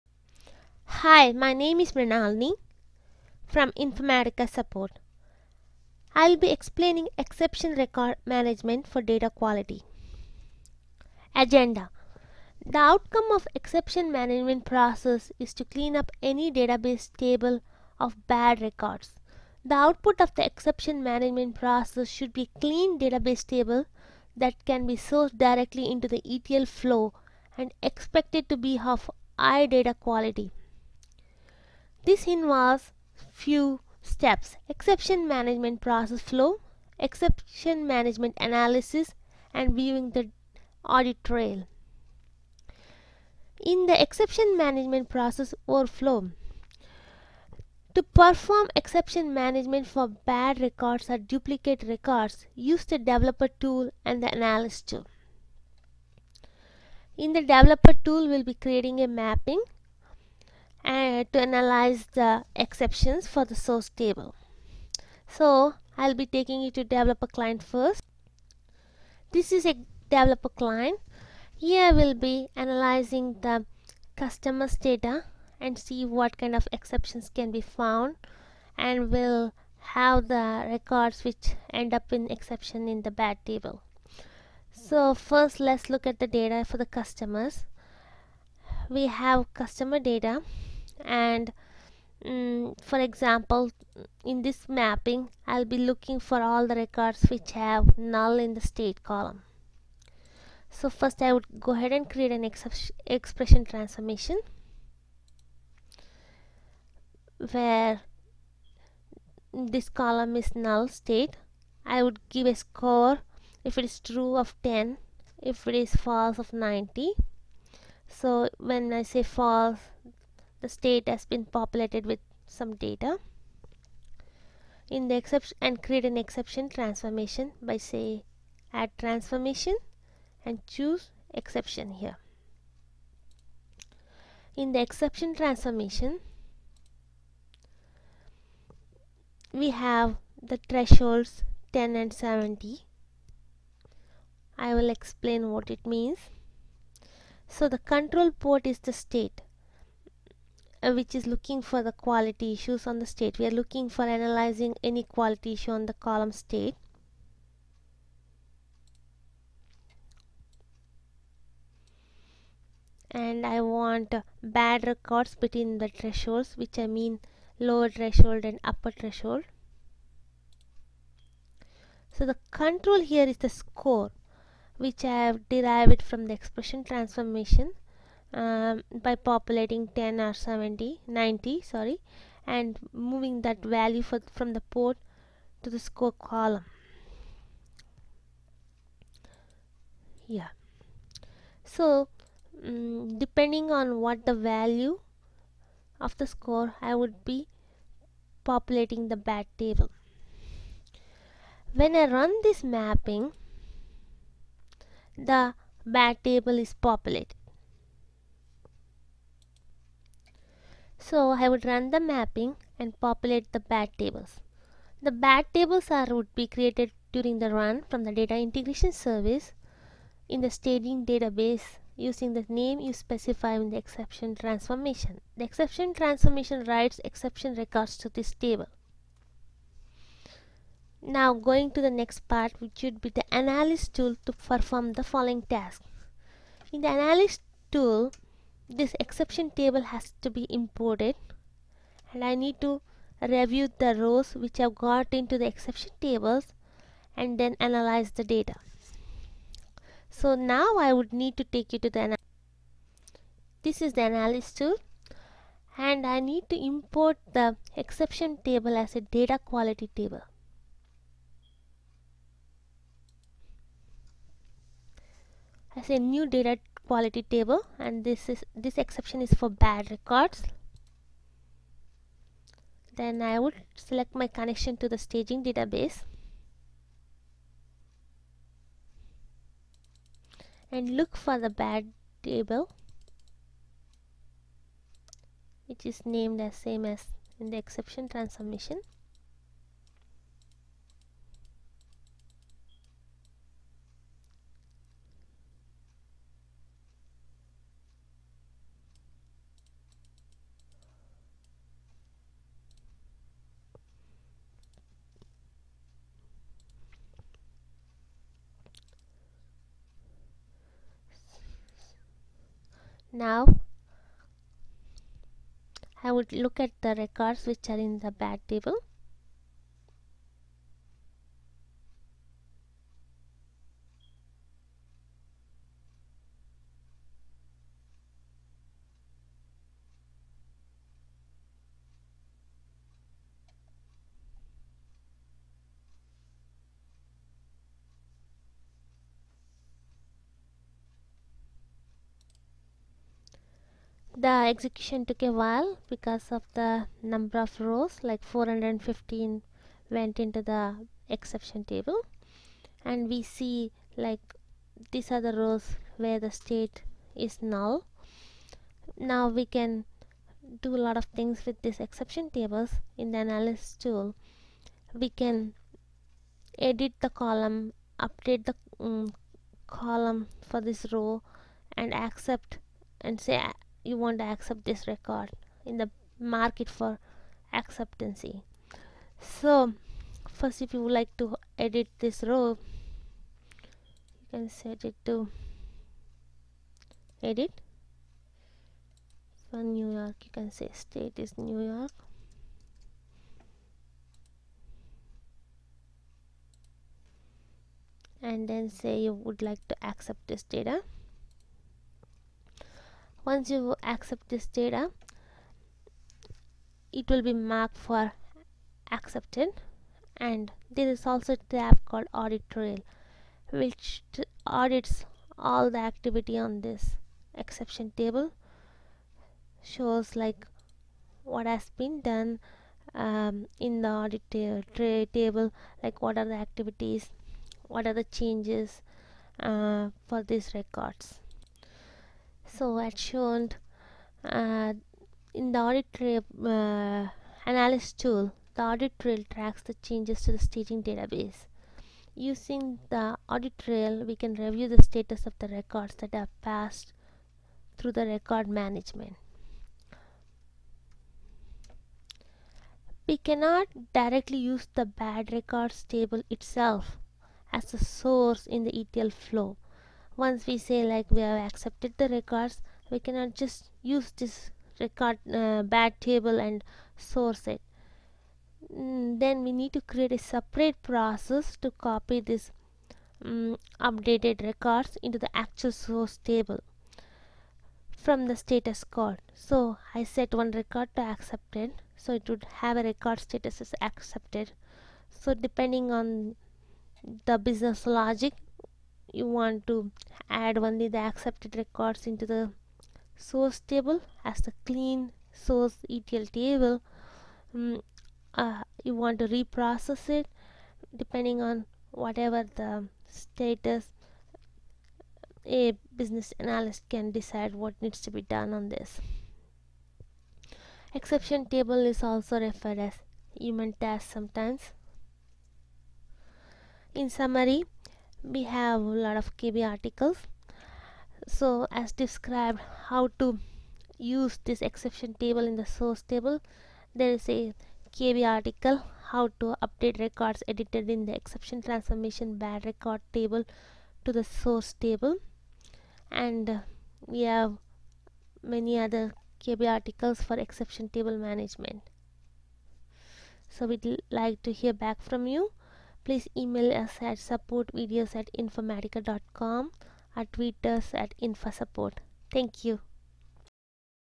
Exception Record Management
The outcome of Exception management process is to clean up any database table of bad records (outliers). The output of the Exception management process should be a clean database table that can be sourced directly into the ETL flow and is expected to be of high data quality. This video explains the same.